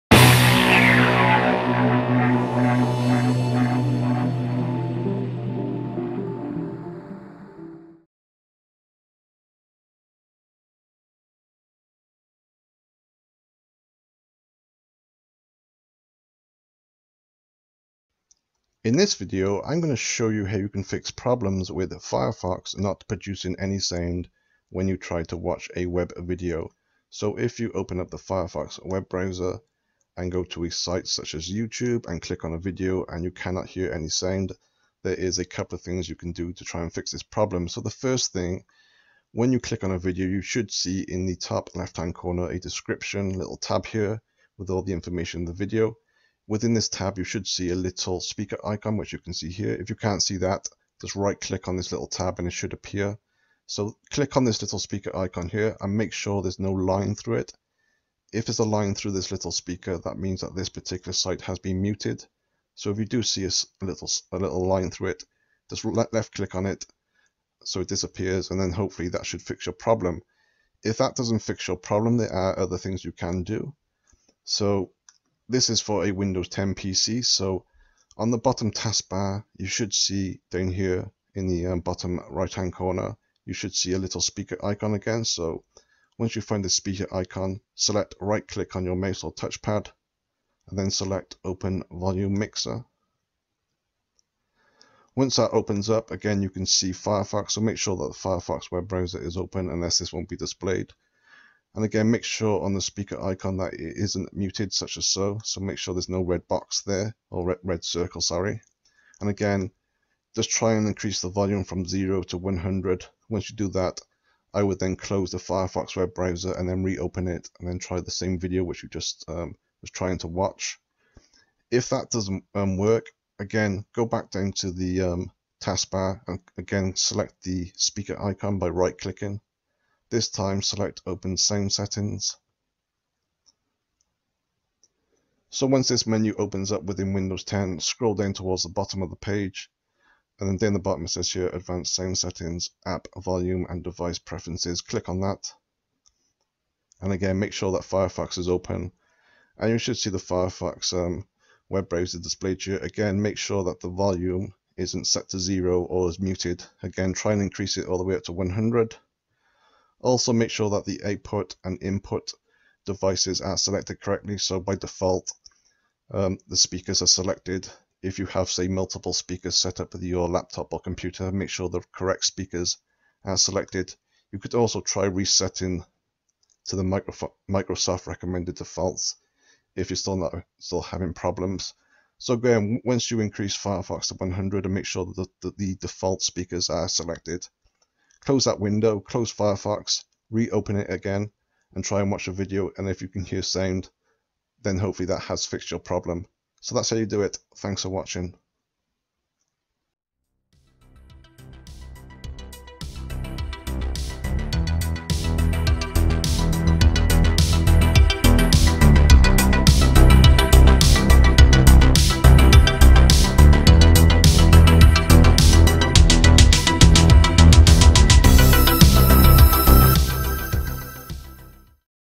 Fix: Firefox Not Producing Any Sound When Trying to Watch a Video
If the Firefox web browser is not producing any sound, there may be a simple fix. First, check that the speaker icon within the browser isn't muted. If that doesn't work, select the volume mixer option by right clicking on the speaker icon on the bottom task bar within Windows 10.